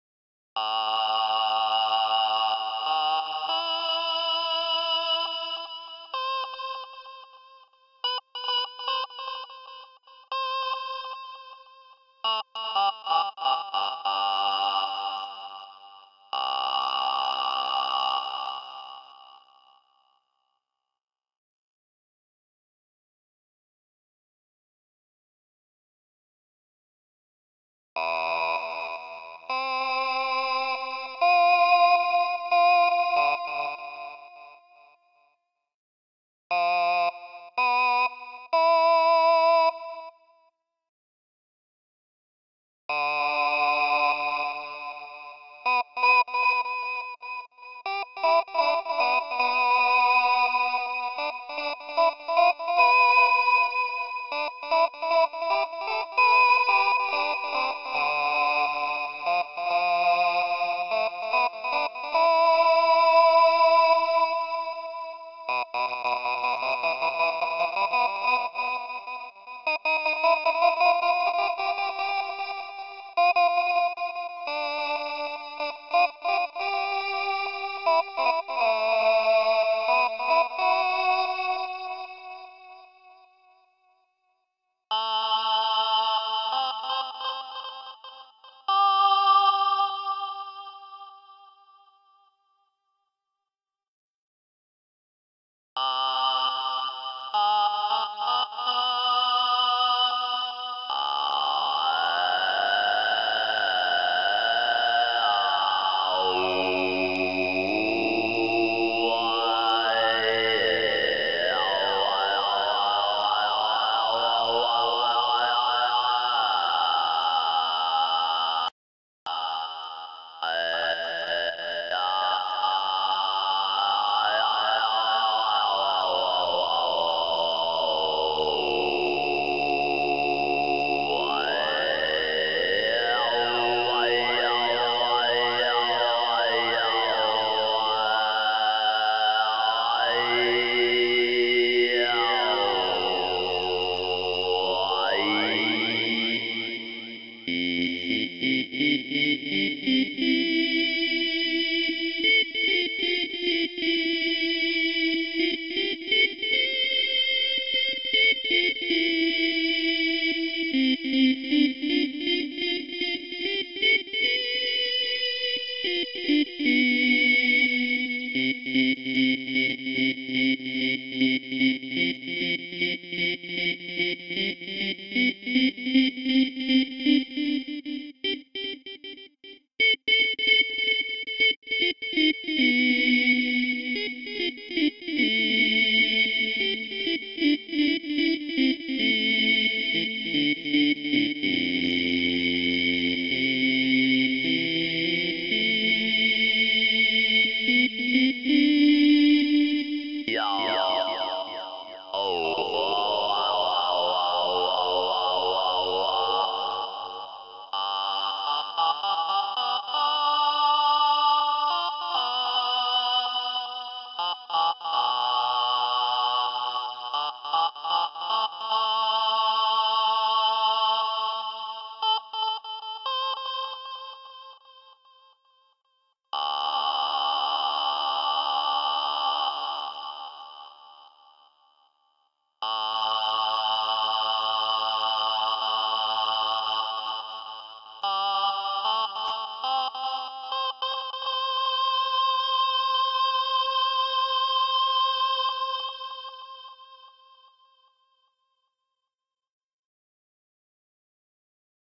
Presentando el Delay Lama en LMMS Descargalo
Este es un viejo instrumento VST muy pintoresco, que conservo desde mis inicios y que sigo utilizando. En LMMS.
Lo puedes descargar gratis en este enlace:
Es un simple archivo DLL, lo copias y lo pegas en tu carpeta de VST y listo.